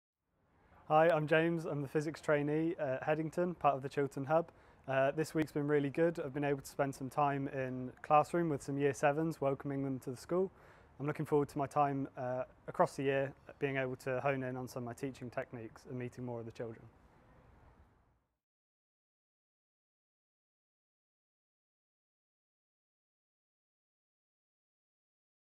Be a Hero, Be a Teacher - James
Meet James, who's one of our 'Engineers Teach Physics' trainees. "This week's been really good," he said, at the end of his first week working with students.
Are you interested in becoming a Physics or Mathematics teacher? to find out more.